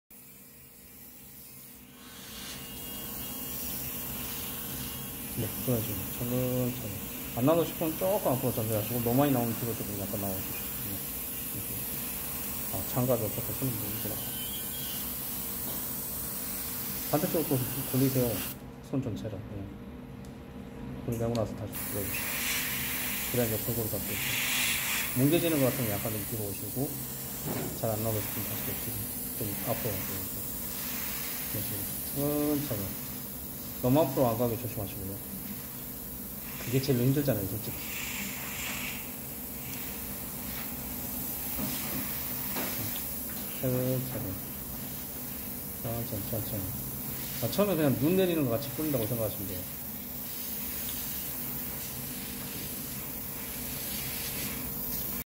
튜닝타임즈 프라모델 강좌 용산팀 OOO님의 Nendoroid Obitsu Custom 넨도로이드 오비츠 커스텀 내가 키운 S급 Han Yu-jin 한유진 색칠
용산 아이파크문화센터 넨도로이드 오비츠 커스텀 수업 수강생 OOO님이 머리 부품 색상을 검정색으로 바꿔가며 에어브러시로 색칠해주고 있는데요, 검정색 락카 도료에 락카신너를 4:6 정도로 묽게 희석하여 살살 여러번 칠하면서 덮어가고 있습니다^^ 튜닝타임즈